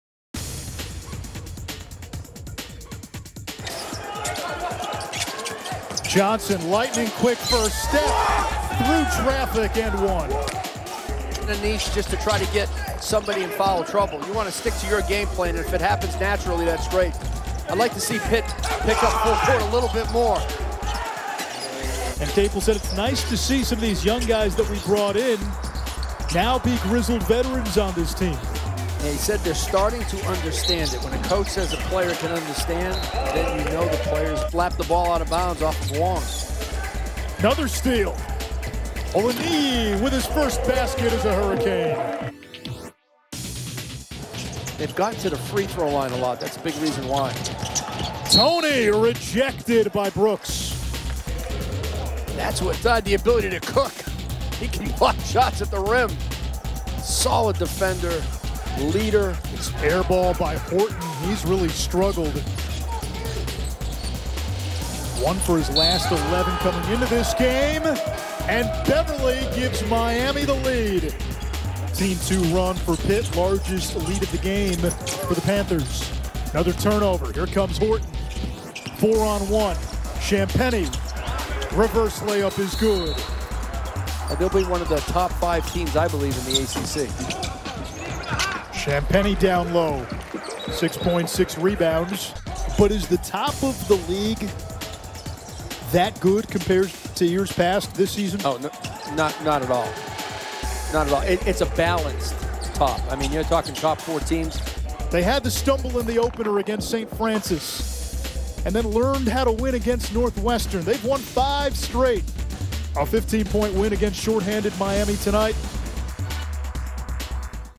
Pittsburgh vs. Miami Men's Basketball Highlights (2020-21)
Pittsburgh vs. Miami: The Panthers went on the road and got a 70-55 win over the Hurricanes, who were missing several key players. Ithiel Horton led Pitt with 18 points and Au'Diese Toney chipped in with 15 points. Isaiah Wong paced Miami, scoring 21 points on 6-10 shooting.